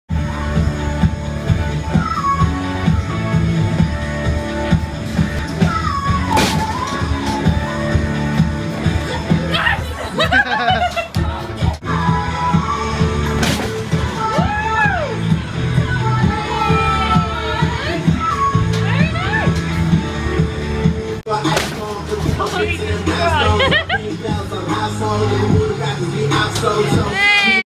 Punching Game at the Arcade - Girl, Boy, Man
Laura, Sam and Byron all have a go at punching and all score over 9000!

Sam Reeves
Laura Reeves
Byron Ovenstone